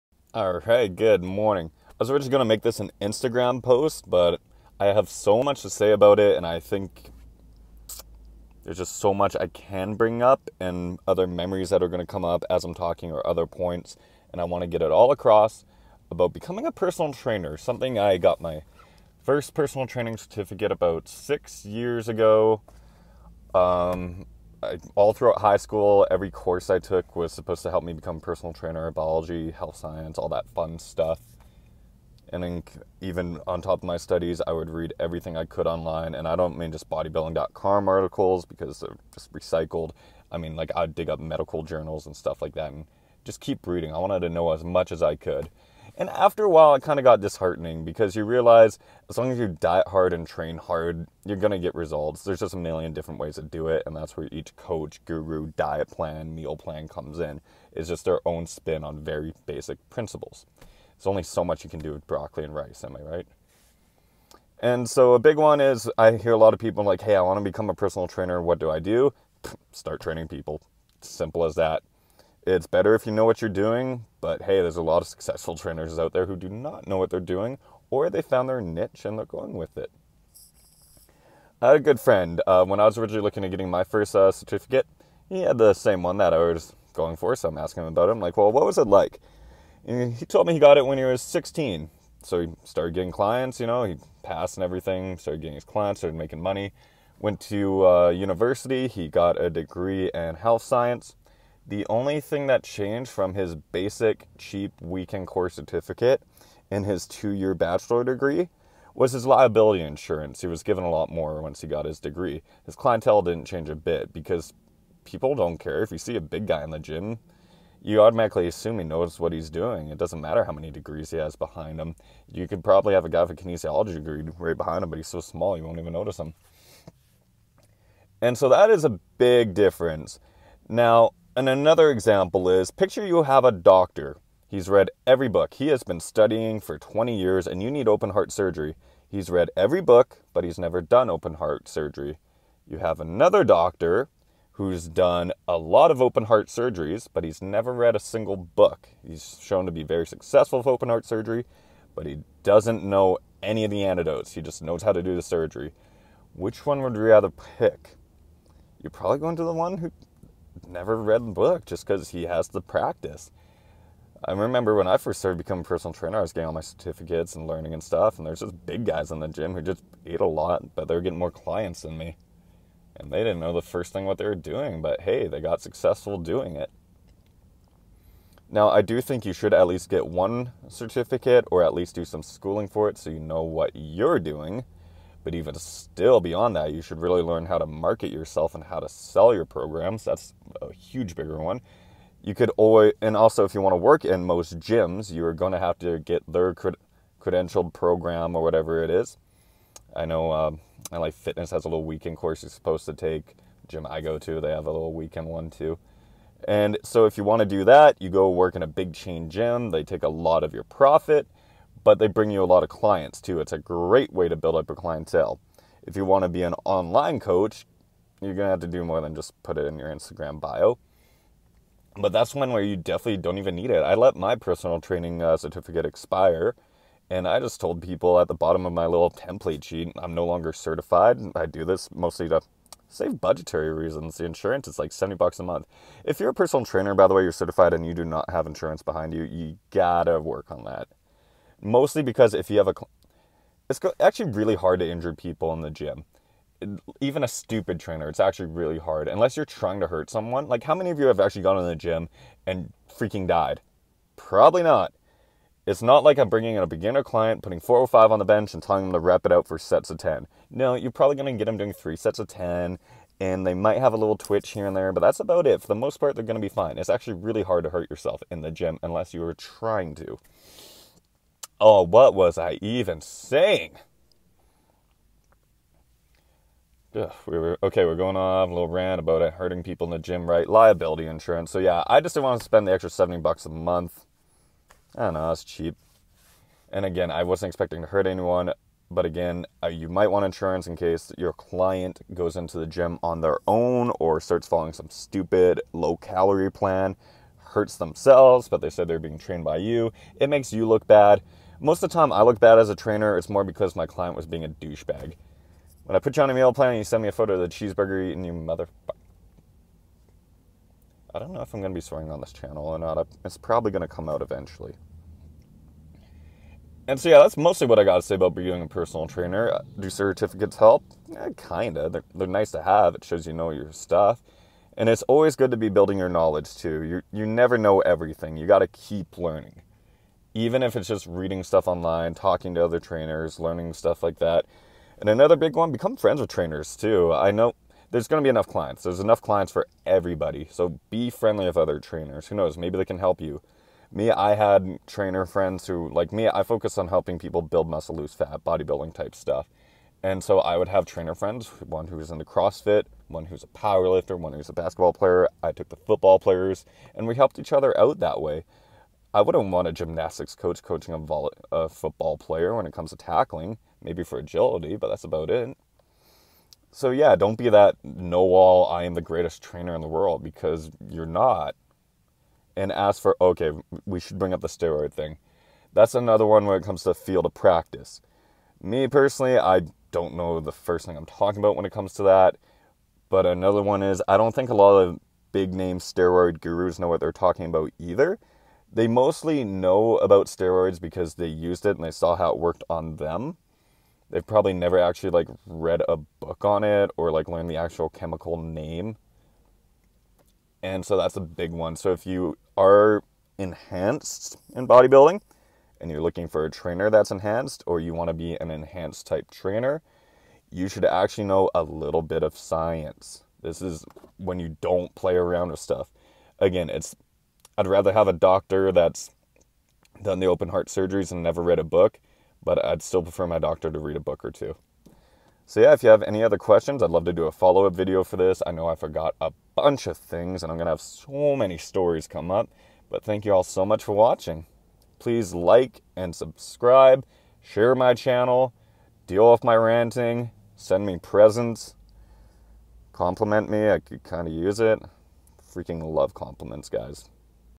Truth About Becoming A Personal Trainer
I tell how to become a trainer, get clients and cover the things you wouldn’t expect.